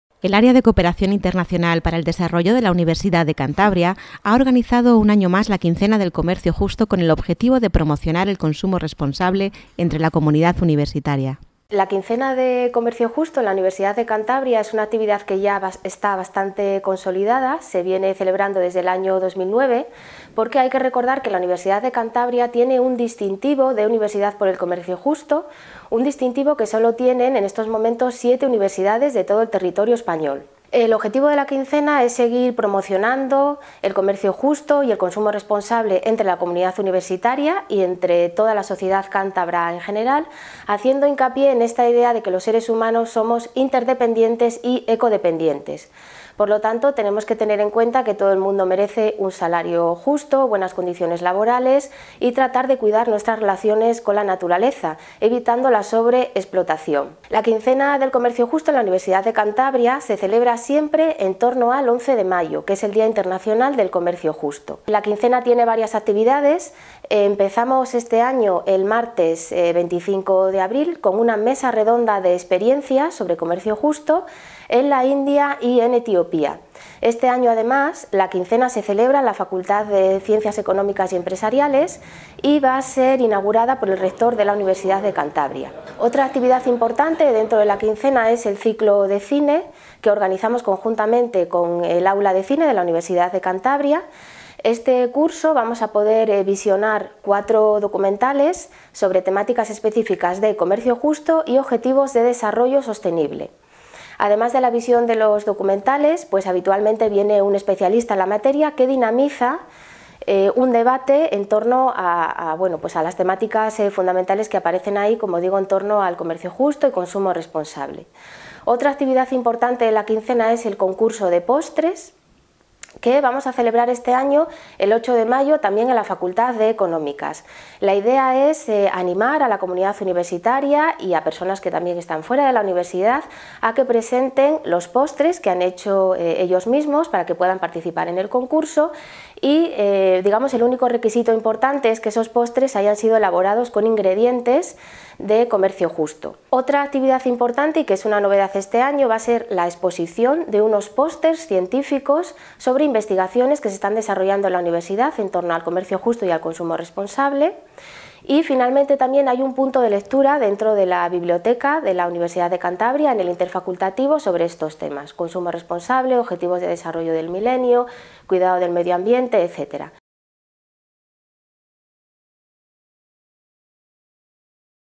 Quincena del Comercio Justo en la UC
Desde el año 2009, el Área de Cooperación Internacional para el Desarrollo (ACOIDE) de la Universidad de Cantabria, en colaboración con el Grupo Universitario por el Comercio Justo, organiza la Quincena del Comercio Justo con el objetivo de promocionar el consumo responsable entre la comunidad universitaria, en particular, y la sociedad cántabra, en general.